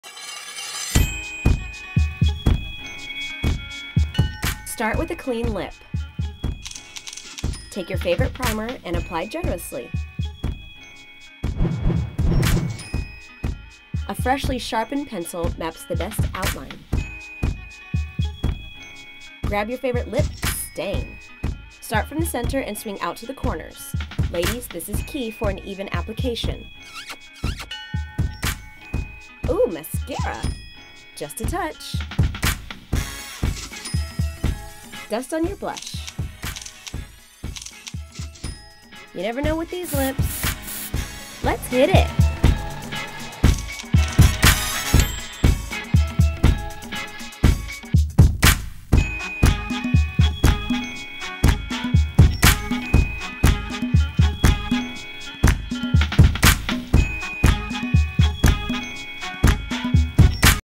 GIOGO DIY "Perfect Strawberry Lip"
What girl doesn't like to wear a red lip all day long? Well, we've found a way and it's a total GC (guaranteed compliment)! Follow these easy steps and make a great impression to all.

The first 10 people who answered the question below correctly, will receive the featured KISSING ELIXIR.
(For reference only) What is the key to an even application? (GIVEAWAY is closed)

Directed & Produced by GIOGO Girls
Styled by GIOGO Girls
Edited by Julianne Waters
Marketing by Tiana Brown